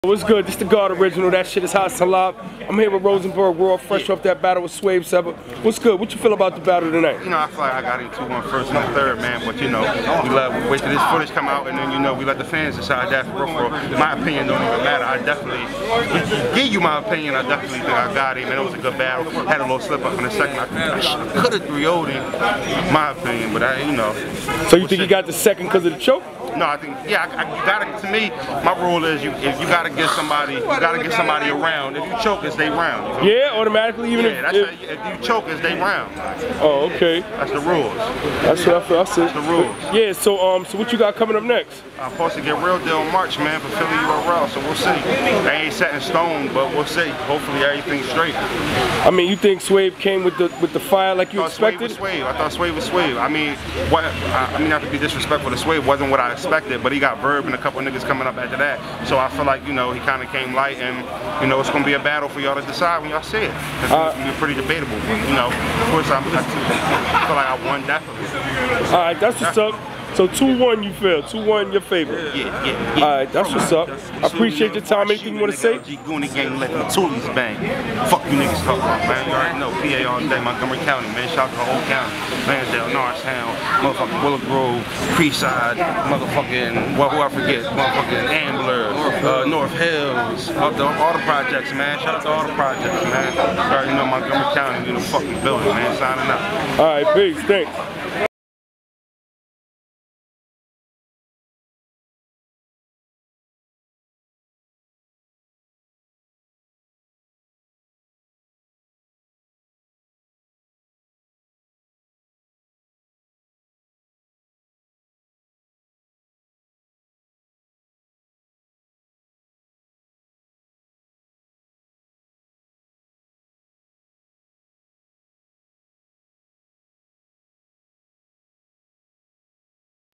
Rosenberg Raw vs Swave Sevah - Rosenberg Raw Gives His Recap On His New Year Order Battle with Swave
Rosenberg Raw vs. Swave Sevah Headlined The New Year Order Battle card, we caught up with Rosenberg Raw to see how he feels he did. See Swave's Recap.